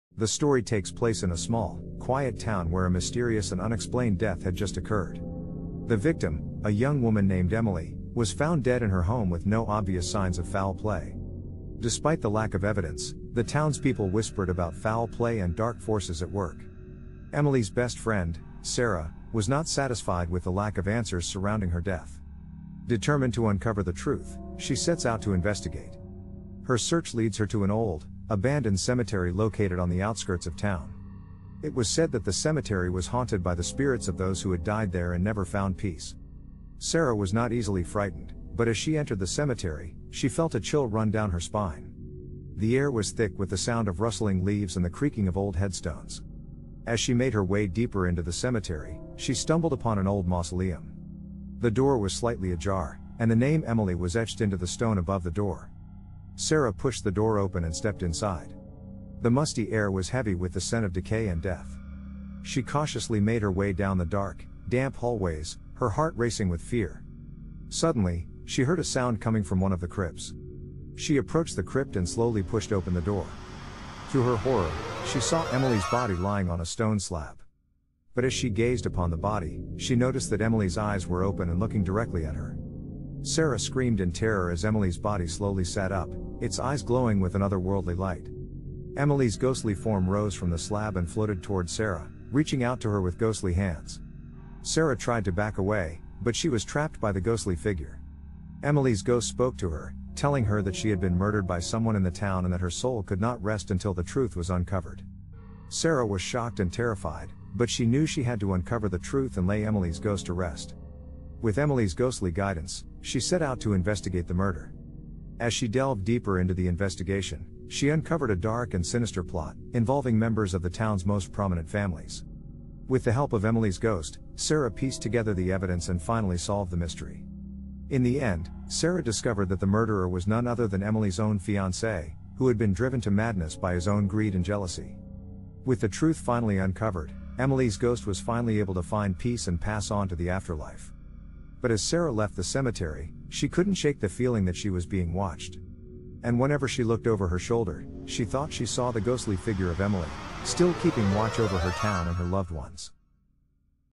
Beyond the Grave: A Short Horror Story
Step into the unknown with "Beyond the Grave," a chilling mini horror story that will keep you on the edge of your seat. This suspenseful video will take you on a journey beyond the limits of the living and into the realm of the dead. With haunting visuals and an ominous soundtrack, this video will send shivers down your spine. Watch at your own risk.